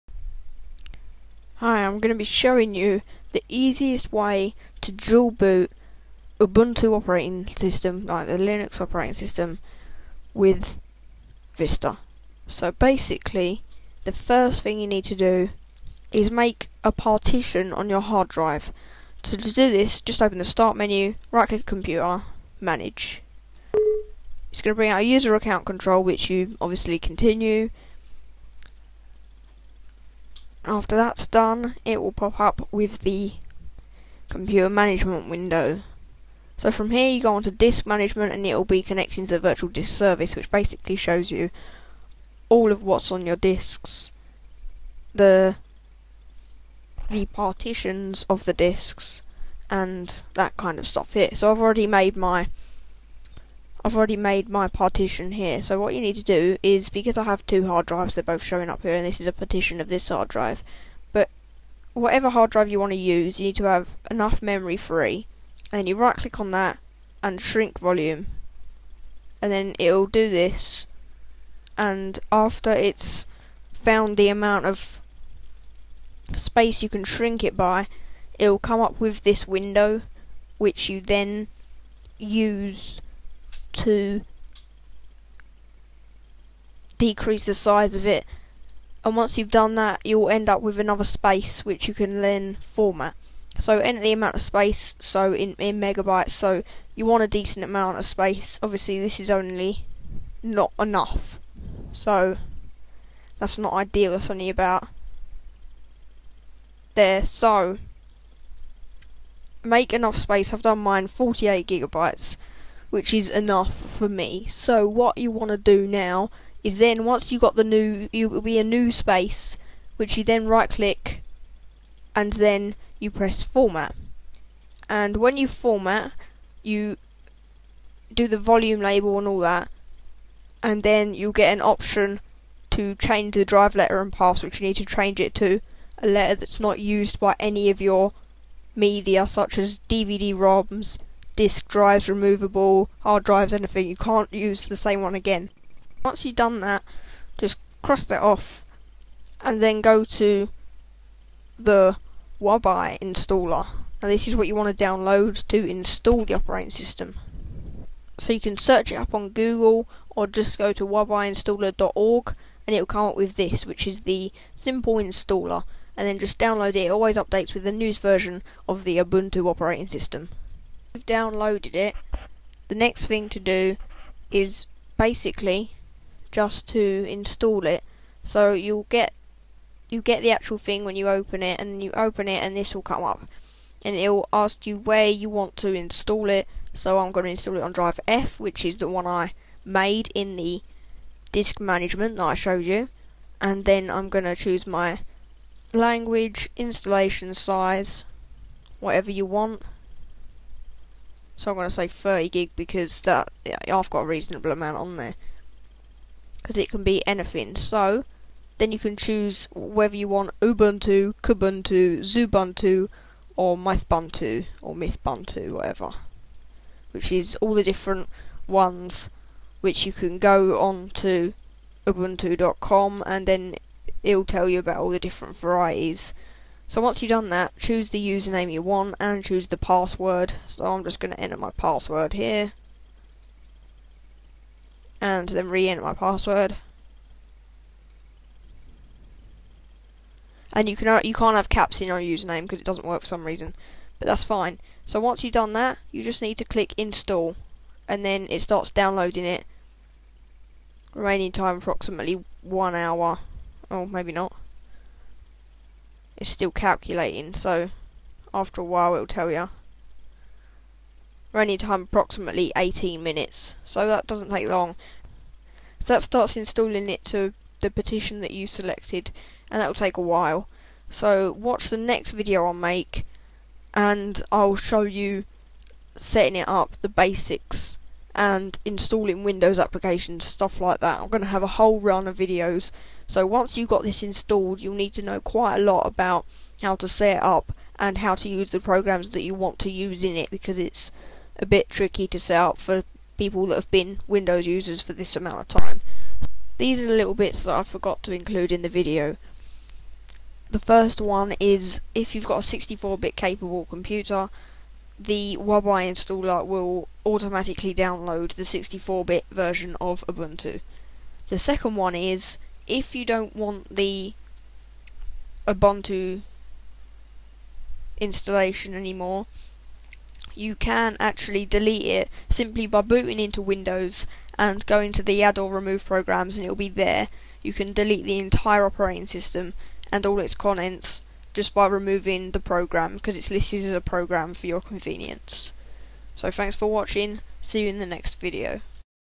Dual boot Vista and Ubuntu easily with Wubi
Dual boot vista and Ubuntu easily with Wubi - I show you the easiest and most user friendly way to dual boot vista and the latest version of Ubuntu Linux using a simple piece of software.